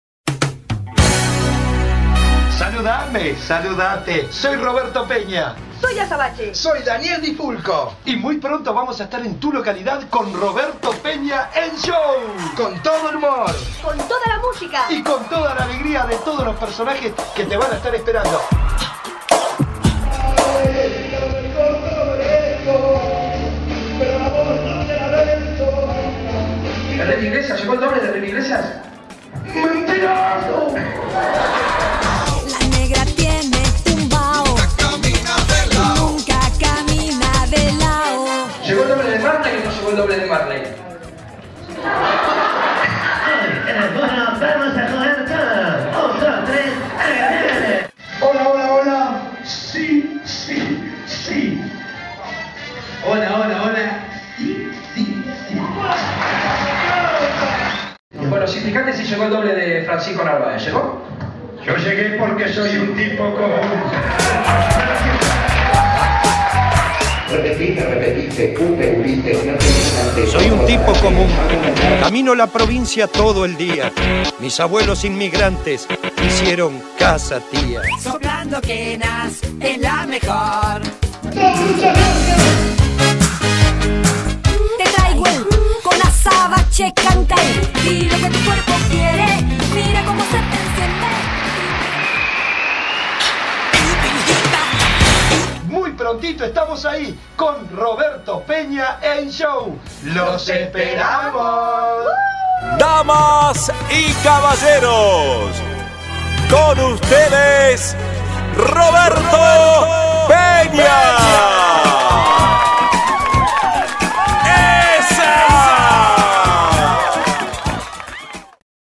Roberto Peña - Contratación Directa - Rapport Producciones
Show e imitaciones - Musical Roberto Peña (ex integrante de Show Match, "Ganador" de Gran Cuñado Político 2009) despliega una galería de personajes donde se encuentran algunos destacados en TV como Alejandro Sanz, Ricardo Arjona, Enrique Iglesias, Michael Jackson, francisco de Narváez y otros.
Acompañado por una cantante, bailarinas y un humorista como intermedio, ofrece una versatilidad que genera aplausos, risas y garantiza un show de primer nivel.